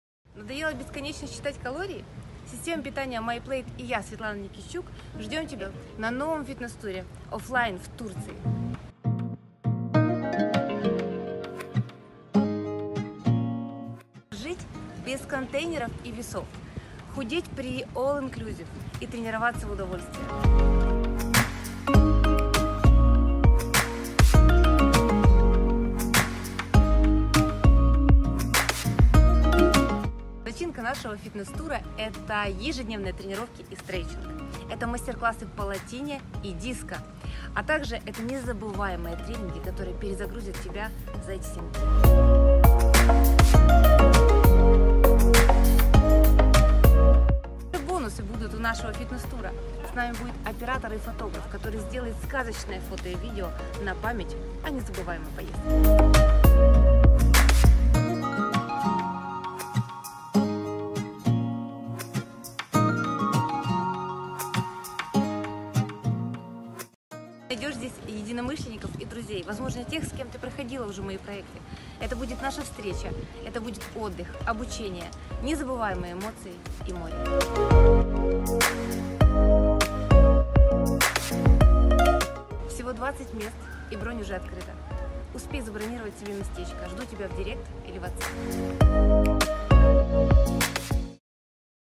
ПРИГЛАШАЮ ТЕБЯ В СВОЙ АВТОРСКИЙ ФИТНЕС ТУР В АПРЕЛЕ 2020 ⚓🐬🌊
Надоело бесконечно считать калории? Система Май Плейт и я - Светлана Никитчук ждём тебя в Турции 30 го апреля 2020 года на новом оффлайн фитнесс туре!
Ты научишься:
👉жить без контейнеров и весов по новой системе питания 🔥
👉худеть при алл-инклюзив🔥
👉тренироваться в удовольствие
Начинка нашего фитнес тура ежедневно:
☀️Тренировки и стретчинг
☀️Мастер классы по латине и диско
☀️Незабываемые тренинги, которые перезагрузят тебя за эти 7 дней.
Бонусы:
С нами будет фотограф и оператор, который сделает на память сказочные видео и фото об этой крутой поездке!
Мы будем все 7 дней вместе, и ты не только похудеешь, но и зарядишься безумной энергетикой, научишься восстанавливаться и эффективно восполнять свои силы.
Ты найдёшь здесь единомышленников и друзей, возможно тех с кем уже проходила мои проекты.
Это будет обучение, отдых, встреча и незабываемые эмоции и море💫
Всего 20 мест🔥🔥🔥
И бронь уже открыта. Поторопись забронировать себе местечко - жду тебя в Директ или в ватс ап ☀️
Отель Viking Star 5* все включено
Для Украины 600$ с перелётом
Для России 690$ с перелётом.
Имейте в виду, что стоимость билетов может варьироваться и цена может немного плавать😊 ❤️ поэтому забронировать можно по такой низкой цене только сейчас по 30% предоплате! Торопитесь 🥰🥰🥰

Почему на 1000 калорий я жирею! Физиология похудения!